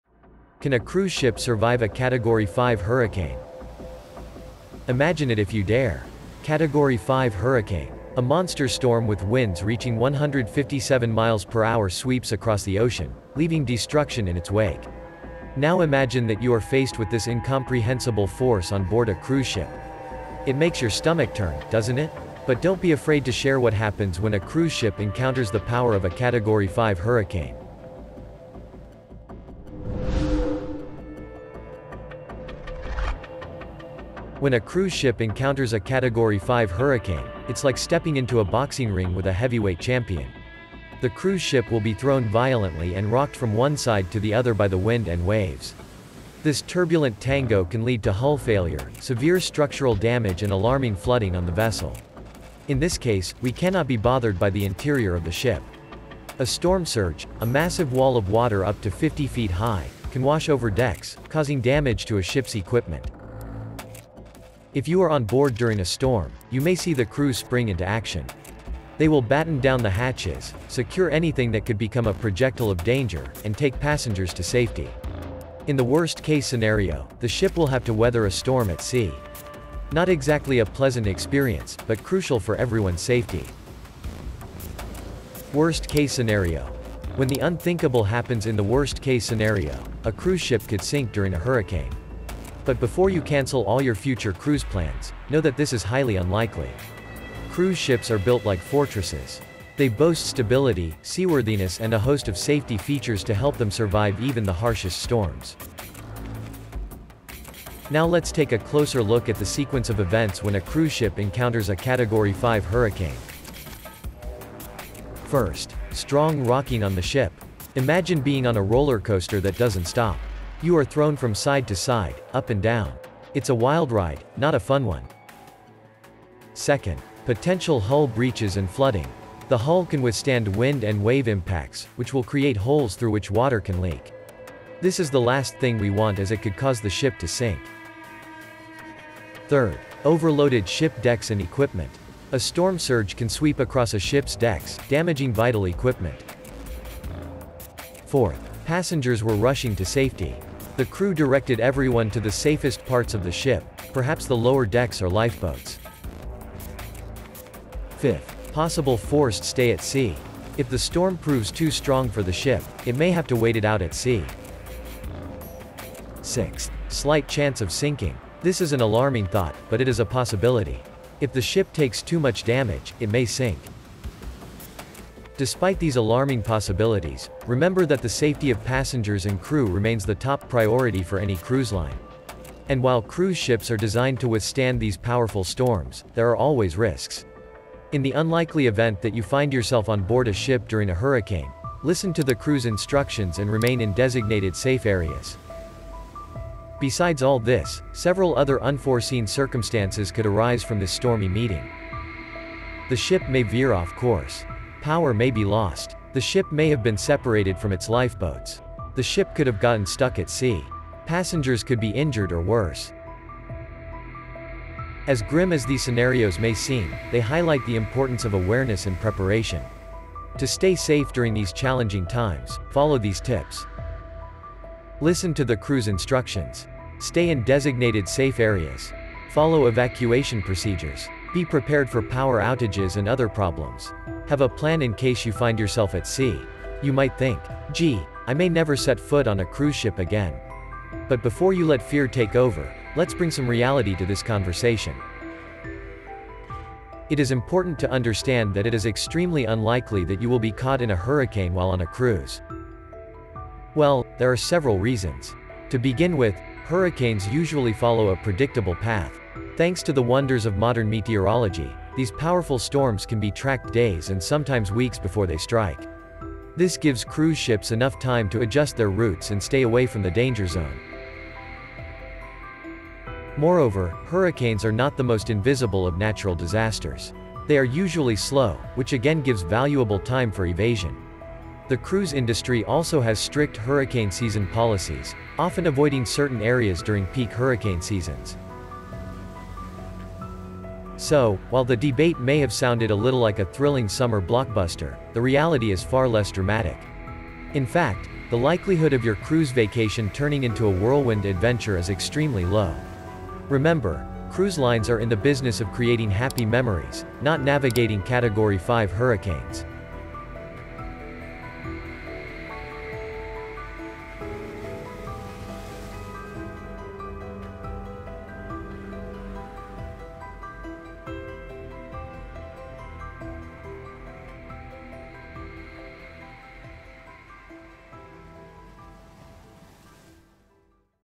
Cruise ship during a Category 5 hurricane | Can the ship withstand
Episodes:
0:00 - Cruise ship in a category 5 hurricane
0:34 - Cruise ship encounters Category 5 hurricane
1:04 - Passengers aboard a cruise ship during a storm
1:25 - Worst case scenario during a storm on a cruise ship
1:47 - When a cruise ship encounters a Category 5 hurricane
2:55 - Passenger safety on a cruise ship
3:16 - Unforeseen circumstances on a cruise ship
3:36 - Dark scenarios on a cruise ship
4:07 - Crew instructions on a cruise ship
4:19 - Meteorological tracking of powerful hurricanes

A monstrous storm, with wind speeds reaching 157 miles per hour, sweeps across the ocean, leaving destruction in its wake. Now imagine that you are faced with this incomprehensible force on board a cruise ship.
When a cruise ship hits a Category 5 hurricane, it's like stepping into a boxing ring with a heavyweight champion. A cruise ship can expect to be violently tossed, rocked and pumped by wind and wave power.